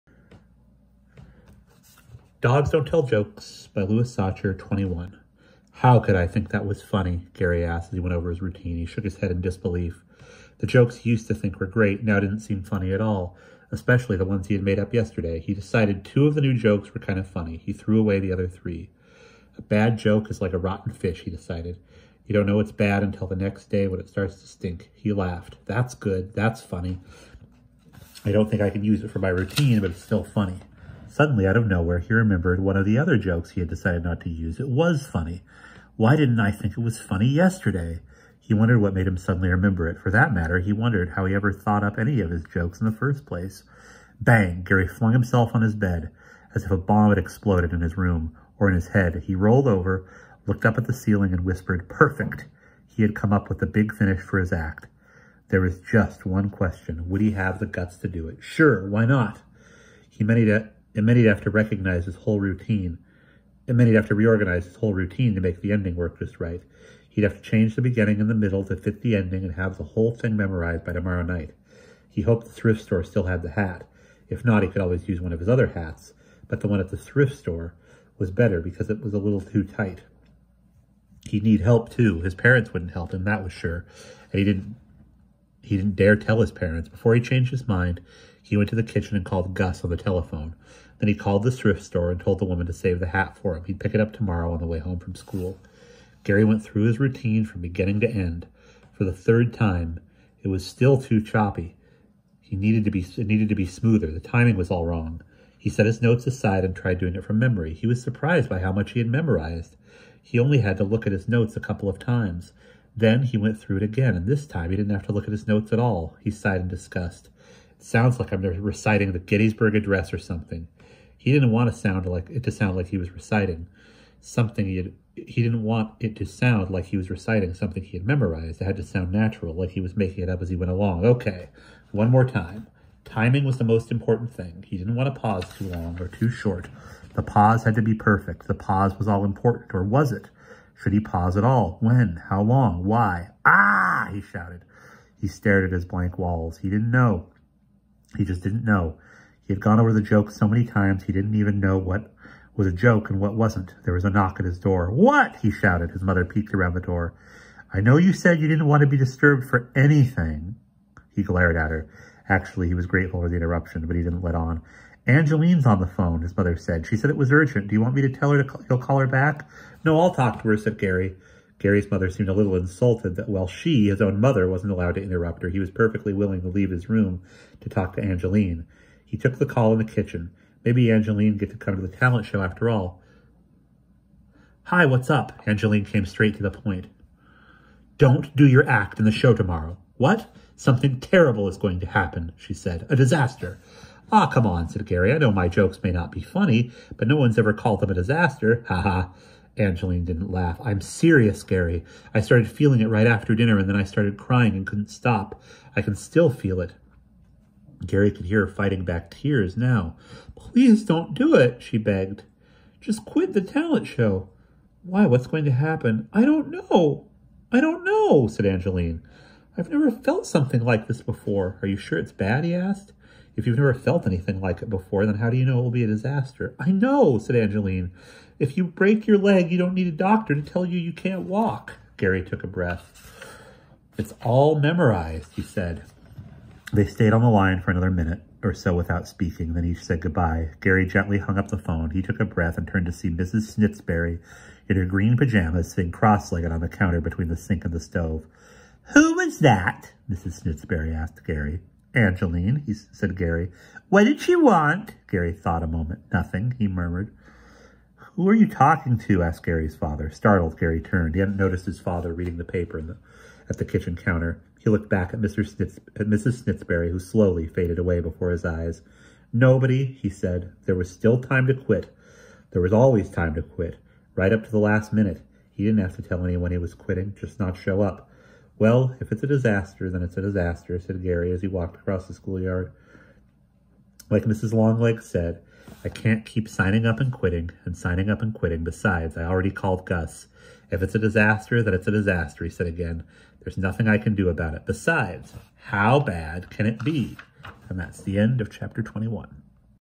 Louis Sachar ‘Dogs Don’t Tell Jokes’ 21.
I read you the twenty first chapter of ‘Dogs Don’t Tell Jokes’ by Louis Sachar